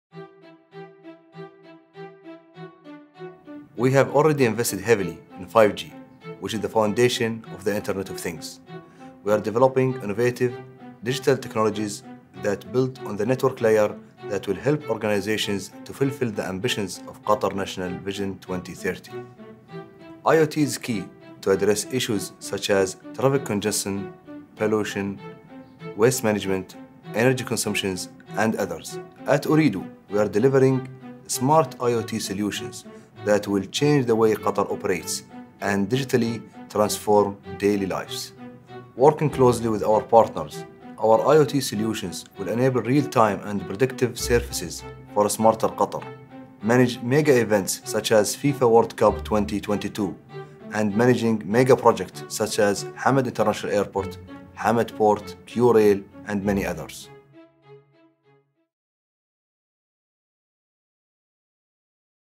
IoT: Revolutionising Ooredoo business services!
Listen to our Chief Business Officer, Sheikh Nasser Bin Hamad Bin Nasser Al Thani, as he describes how the Internet of Things is revolutionising our business services and transforming daily lives.